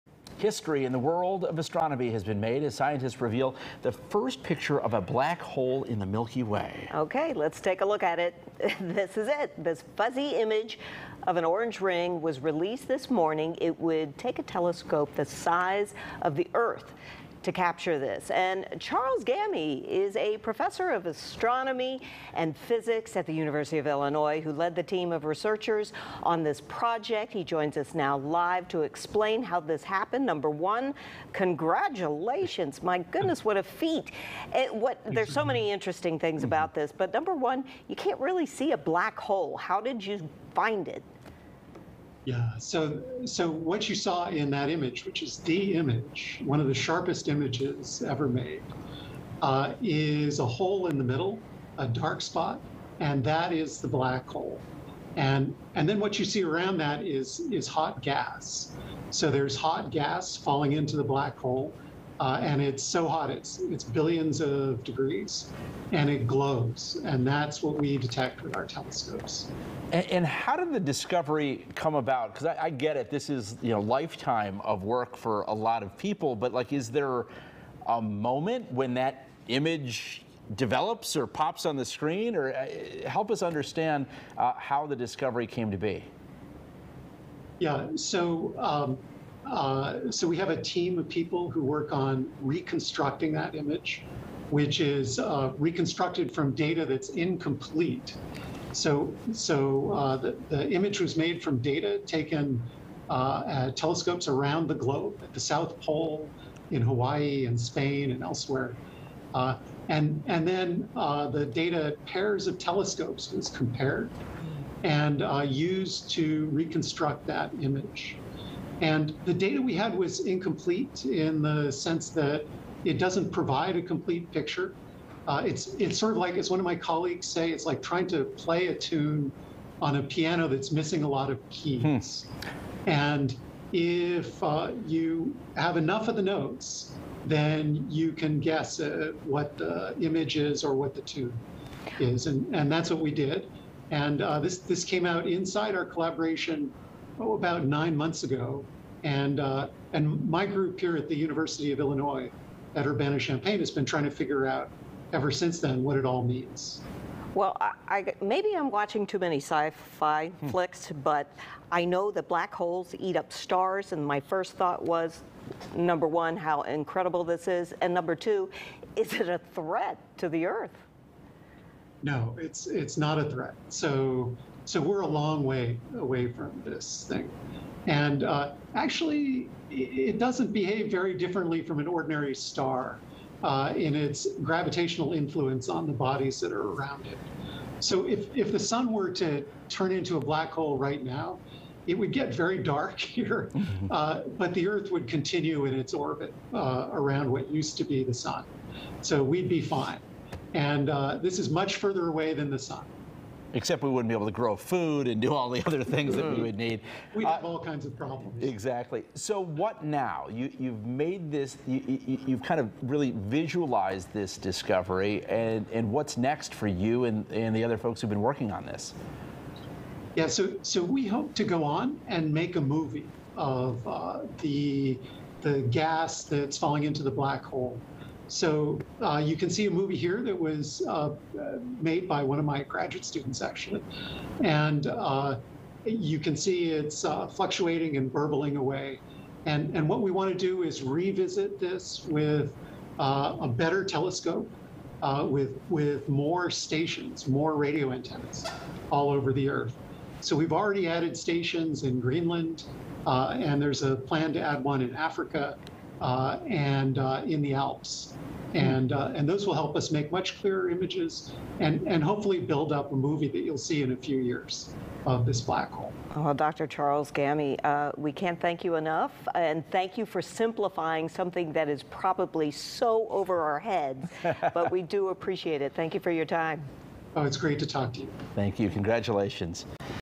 U of Illinois professor talks reveal of Milky Way’s huge black hole
Charles Gamme is a professor of Astronomy and Physics at the University of Illinois. He joined WGN Evening News to discuss the first image of the supermassive black hole at the center of our Milky Way galaxy.